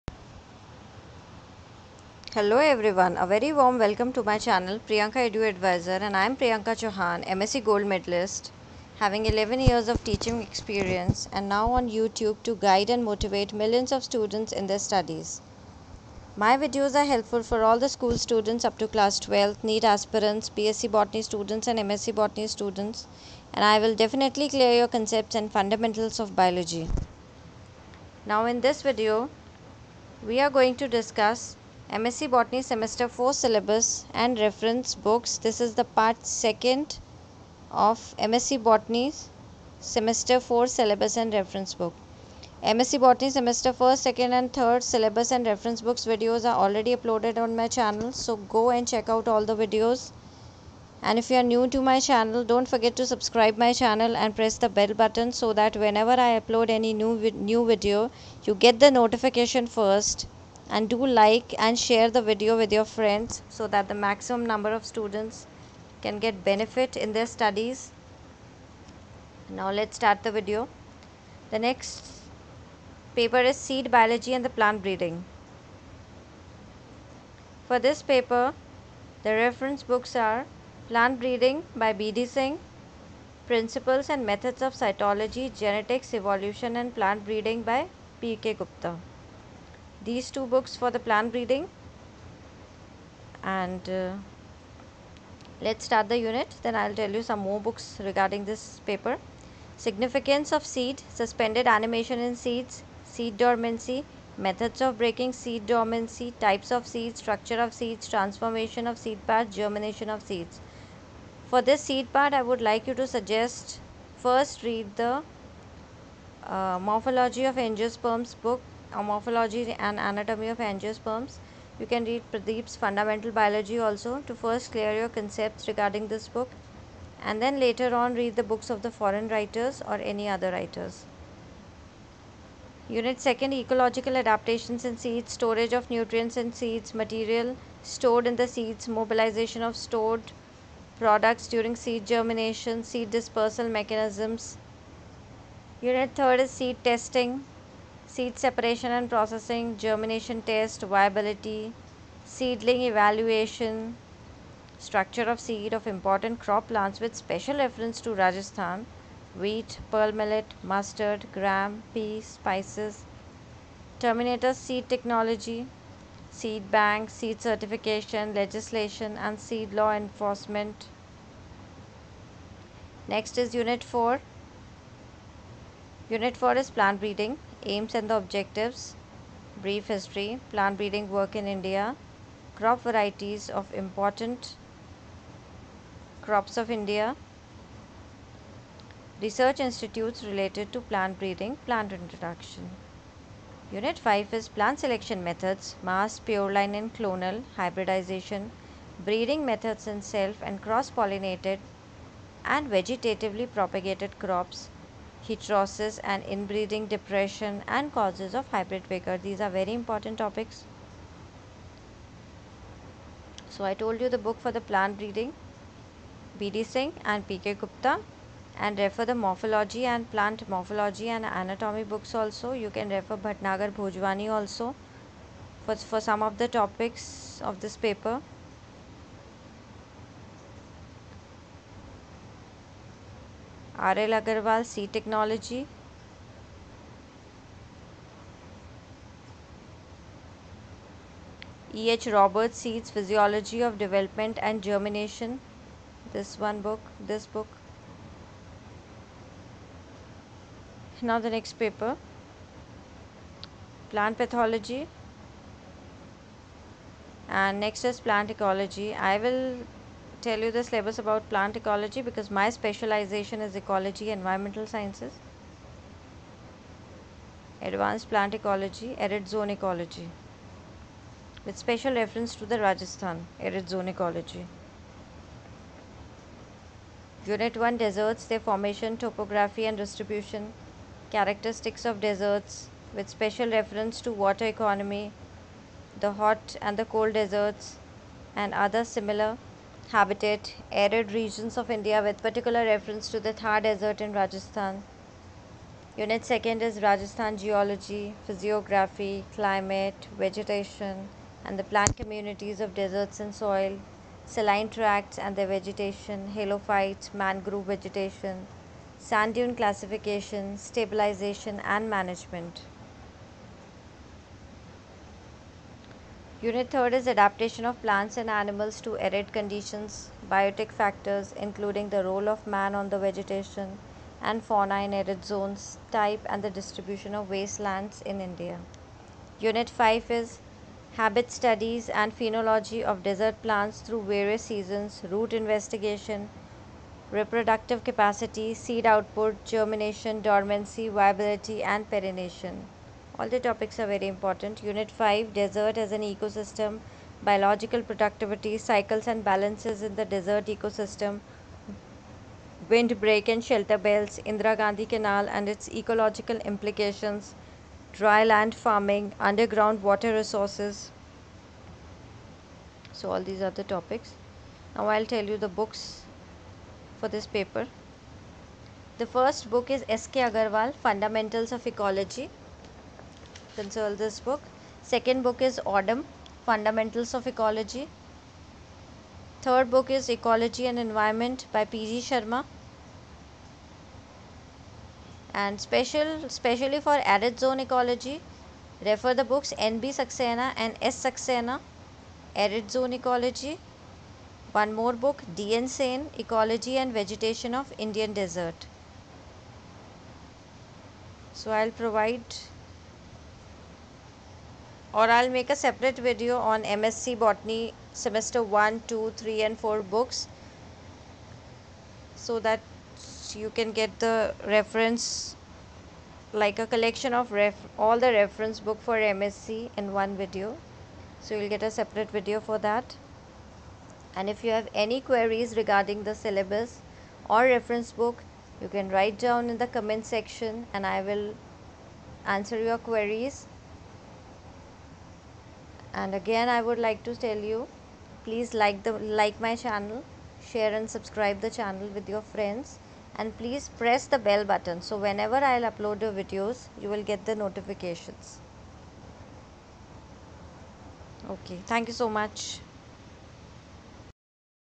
Part-2 M.Sc Botany 4 semester syllabus M.Sc final year#priyankaeduadvisor
This video contains detailed information of M.sc 4 semester syllabus and reference books.